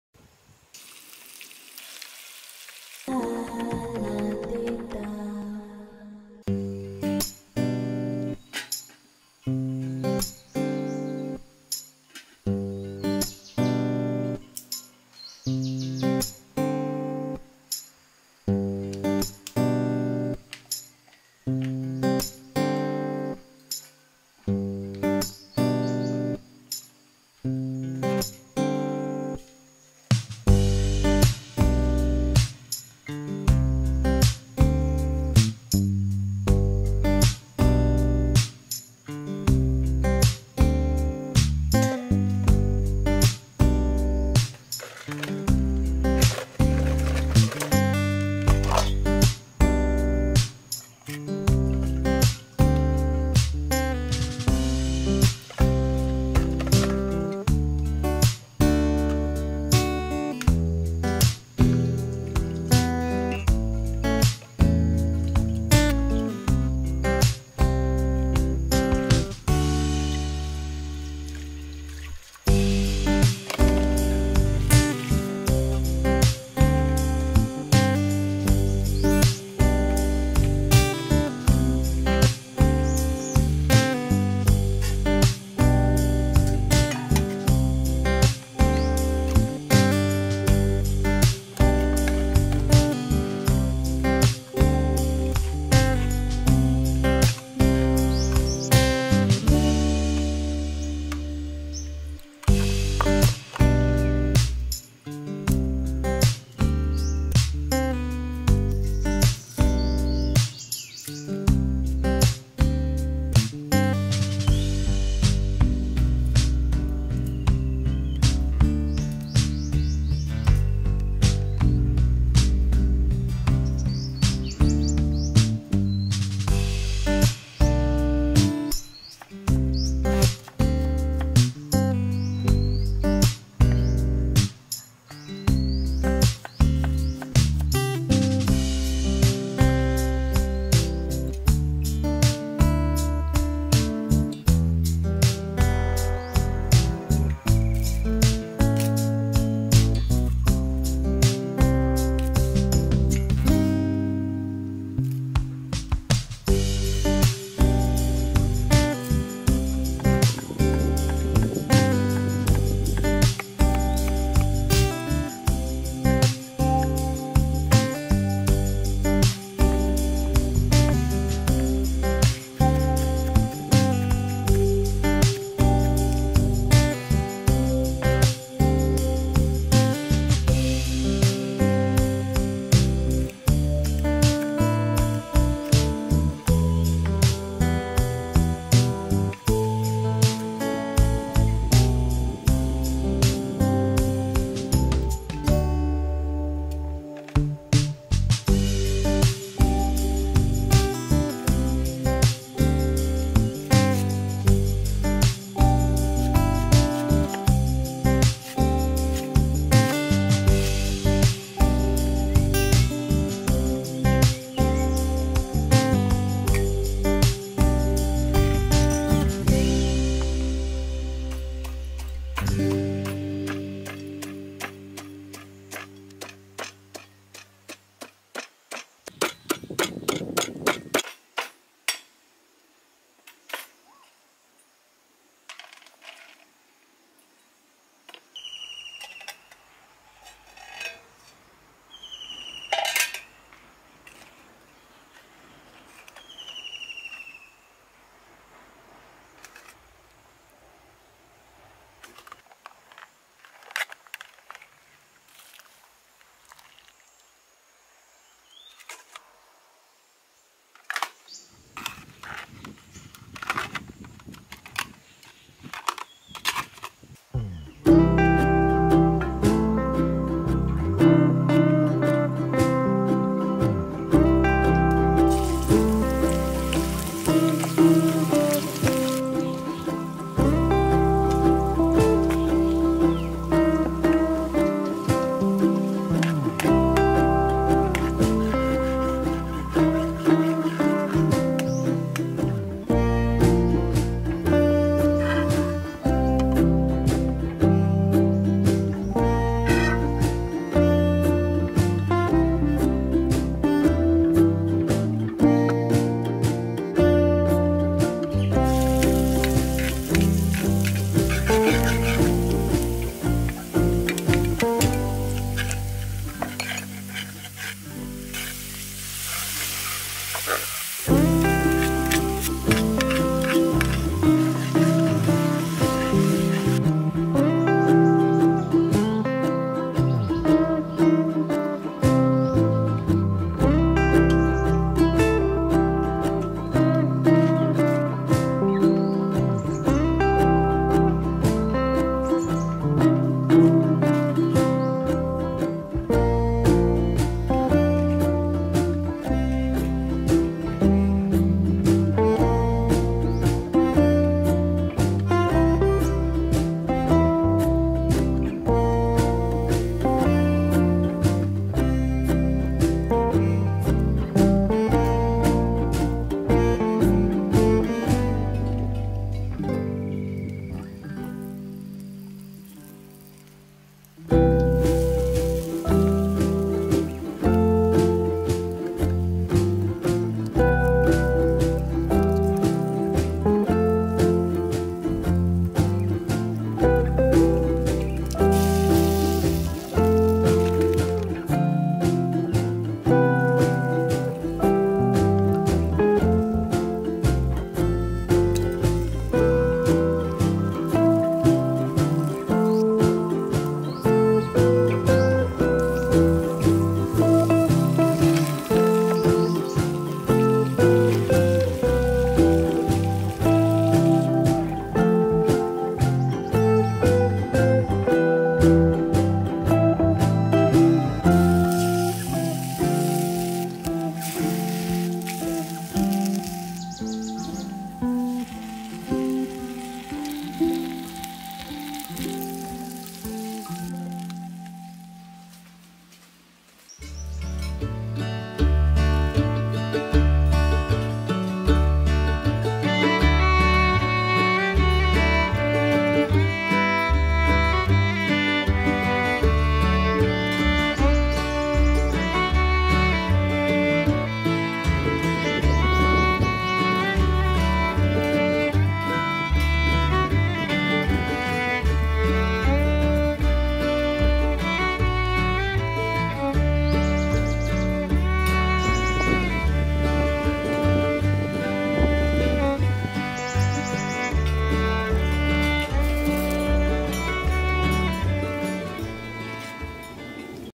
Hong made the half naked pen shell grilled with quail eggs and shallot - a great combination
❤️Welcome to Hong Daily Life #30❤️